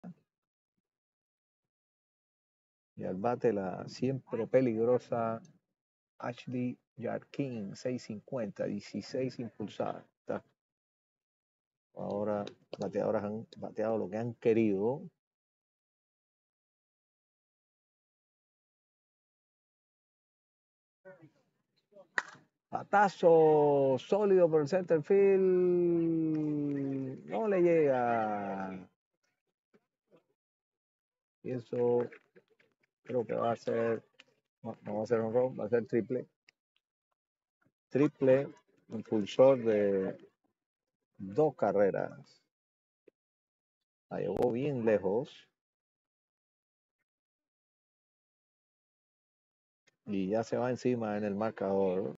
¡Ashley Jarquin Brilla con un Impresionante Triple!
Hoy, Ashley Jarquin deslumbró en el campo con un impresionante bateo que llevó dos carreras al marcador. Observa cómo este talento se convierte en una jugadora clave para su equipo en un emocionante partido.